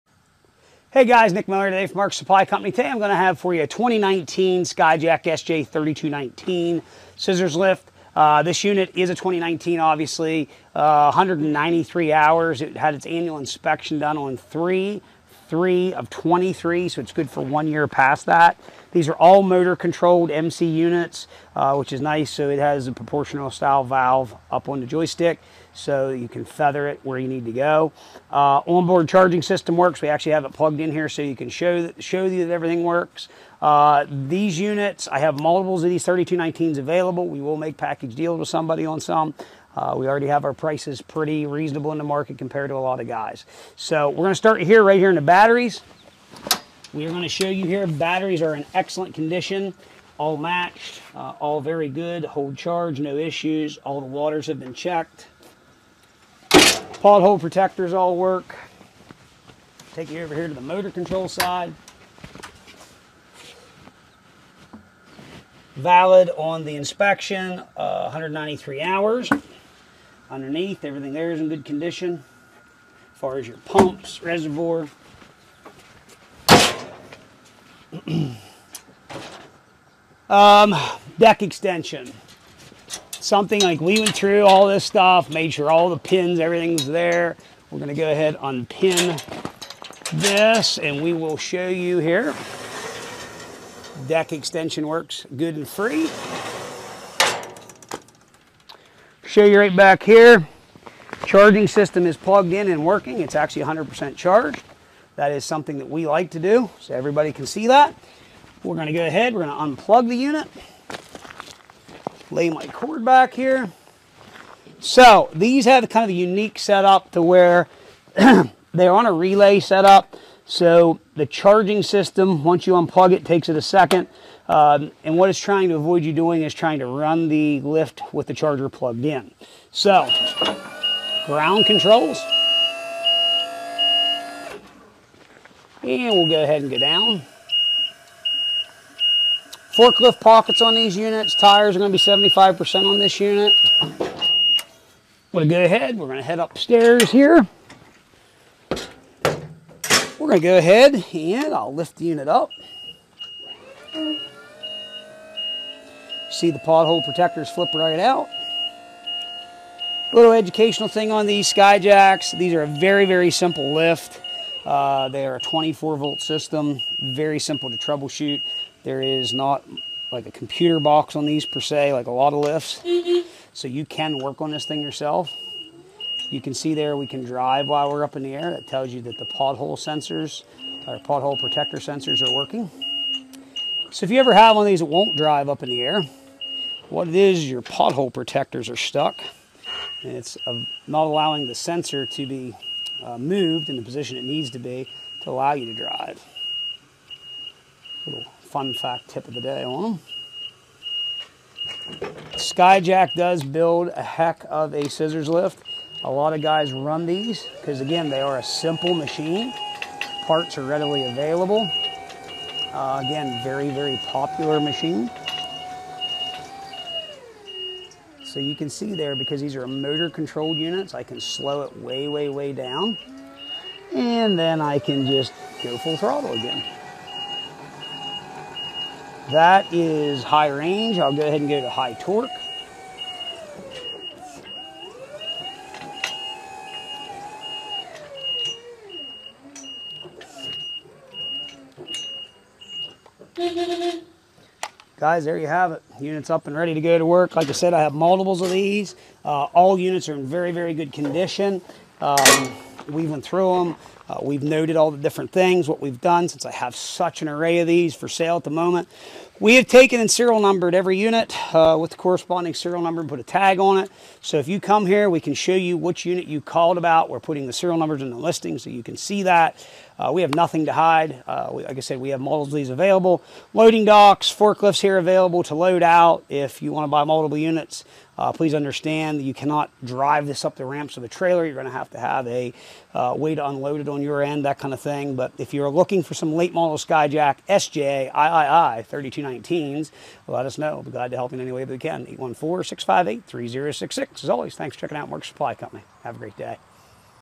2019 SKYJACK SJIII3219 For Sale
2019 Skyjack SJIII 3219 Scissor Lift

193 Hours

Good Safety Inspection Until 3-3-2024

S/N: 22144479

We Have A Bunch Of These For Sale, Please Specify Hours And Serial Number

19' Platform Height

25' Working Height

550 Lbs. Capacity

2 Person

Solid Non Marking Tires In Excellent Condition

64" Long Platform With 36" Extension Deck

Checked The Batteries And They Are In Excellent Condition

On Board Charger

110V Power To The Platform

Fork Pockets

Pothole Protectors

Everything Works Just As It Shoud

Overall Dimensions 70" X 32" X 78.5"

Stowed Ground Clearance 2.375"

Raised Ground Clearance .7"

0 Degree Turning Radius

2 MPH Drive Speed Lowered

0.7 MPH Drive Speed Raised

Please See All Photos And Video

Call Nick Or Matt With Any Questions